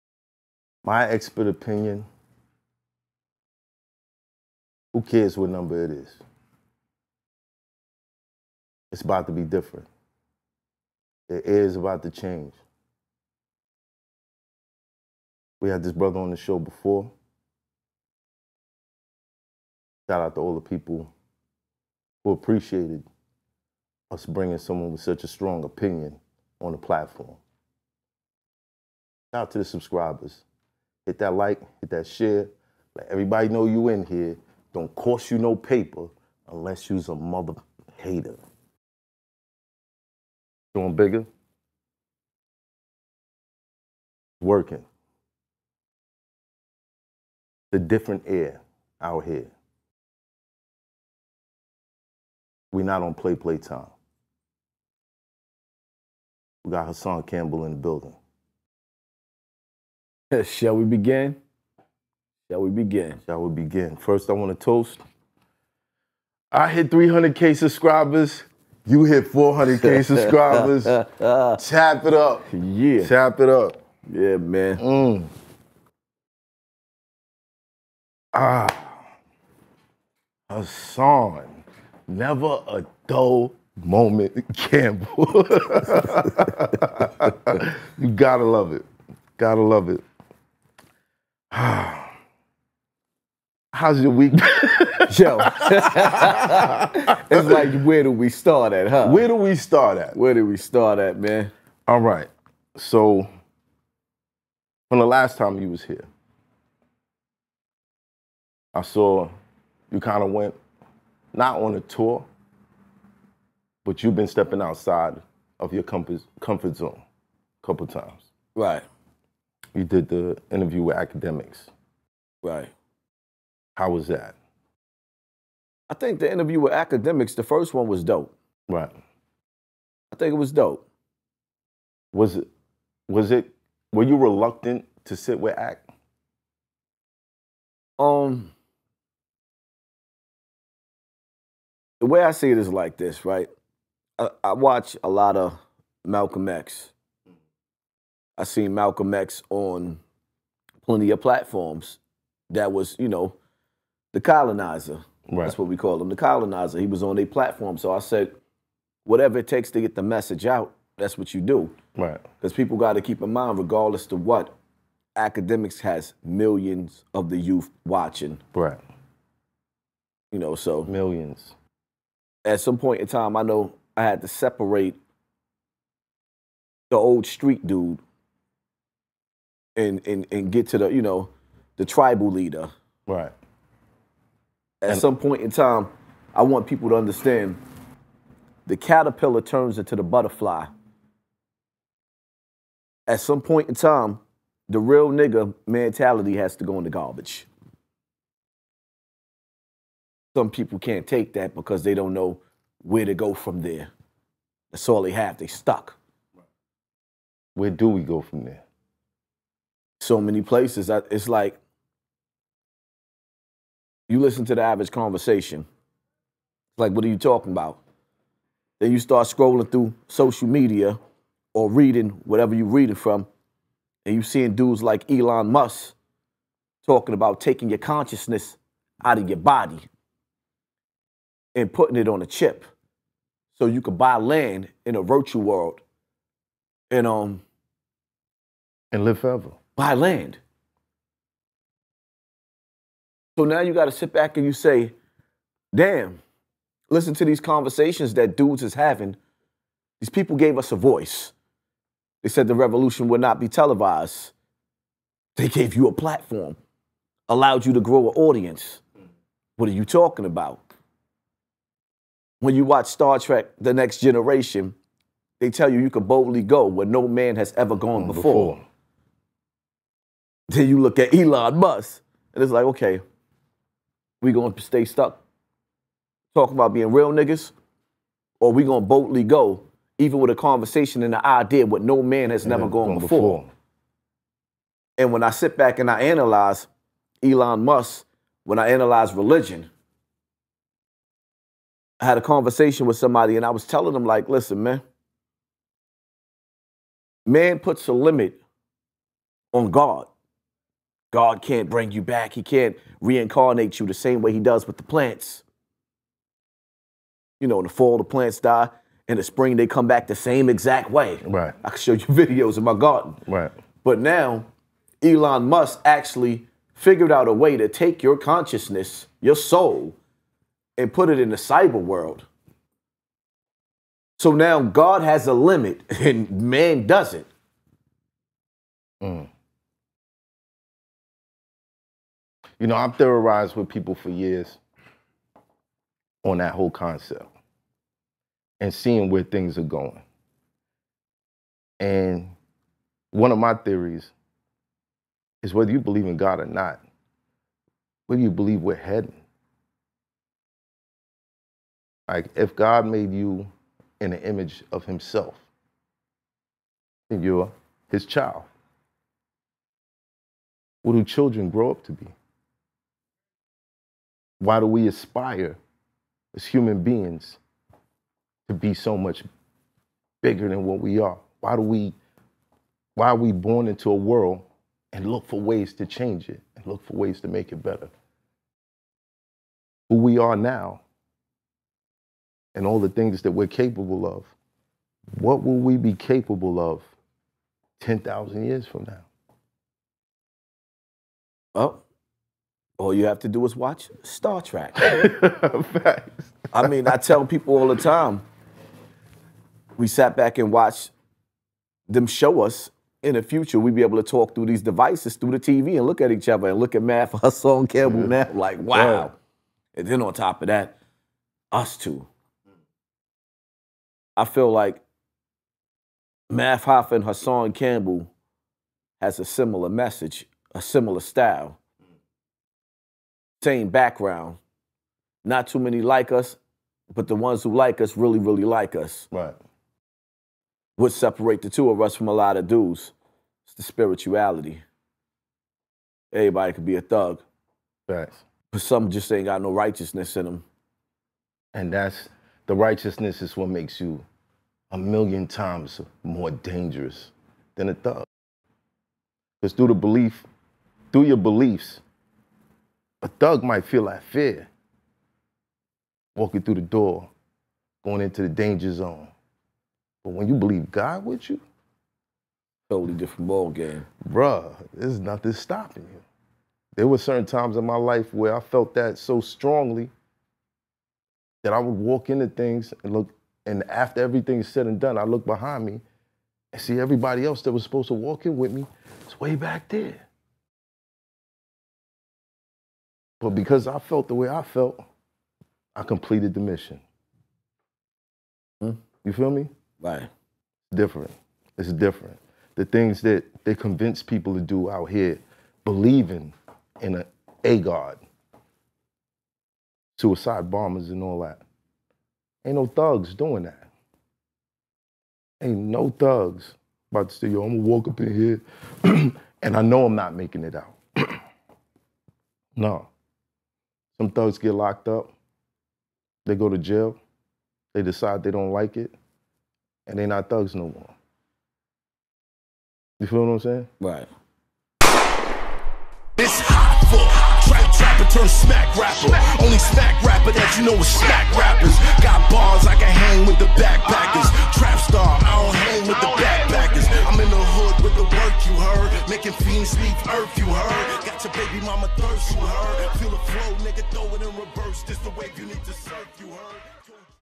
TRUE OR FALSE: "BEING RIGHTEOUS MAKES YOU MORE DANGEROUS THAN A THUG!!!" DROP A COMMENT!!!
BECOME A MEMBER FOR EARLY ACCESS TO MY EXPERT OPINION EPISODES AND NEW SHOWS ON THE WAY!!! SPECIAL GUEST: HASSAN CAMPBELL

❯ PRODUCED BY  @Sp8Ghost ❮